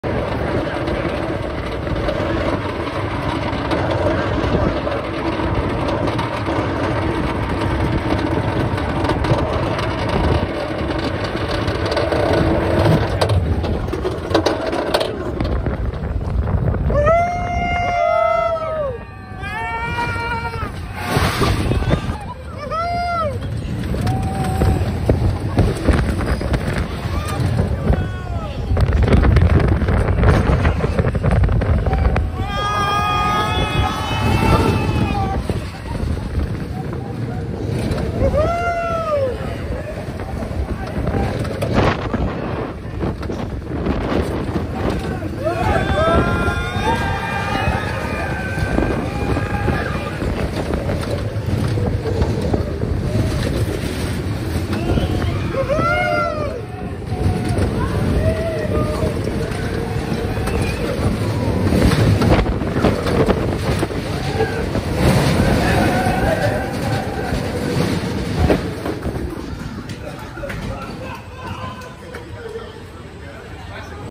Jetline Onride POV Middlerow 4K + 60FPS - Gröna Lund, Sweden🇸🇪
The Classic Schwarzkopf Coaster Jetline! I’m really sorry for the shaking.